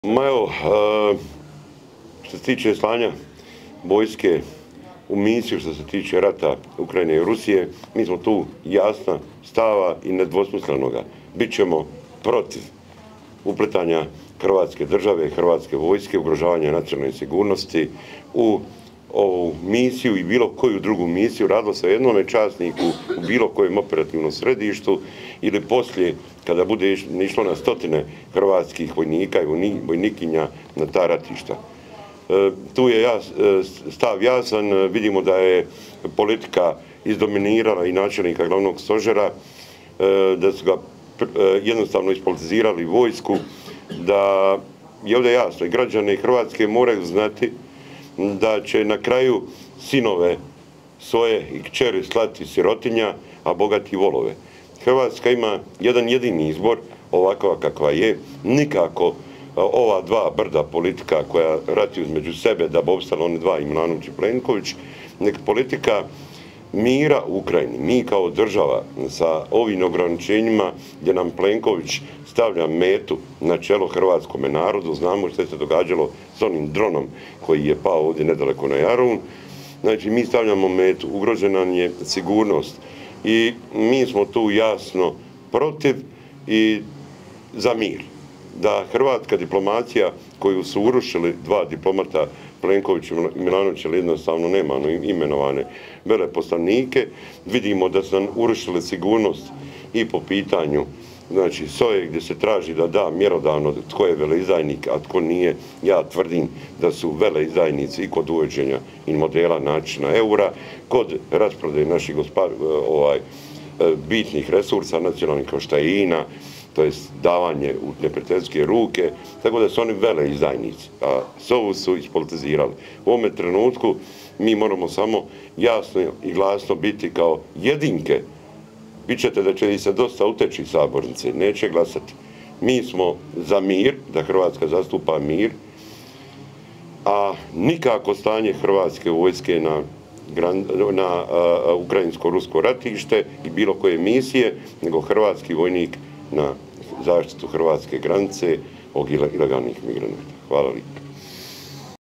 Bulj o slanju vojnika na ratište: "Bit ćemo protiv upletanja Hrvatske vojske u misiju"
Na konferenciji za medije održanoj u Saboru o aktualnim temama govorio je Miro Bulj iz stranke Most.